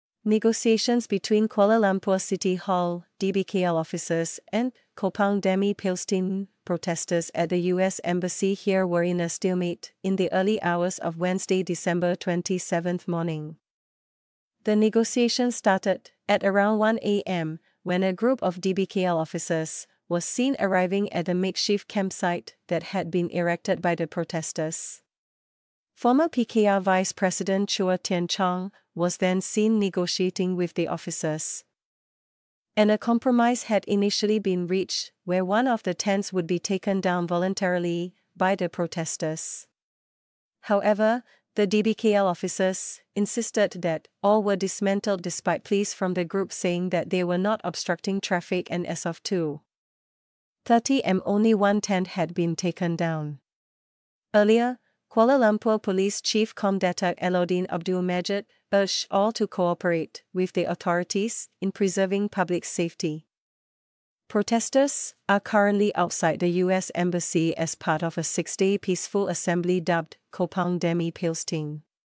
Early morning negotiations between DBKL and Kepung Demi Palestin grind to stalemate
💗📌 Singapore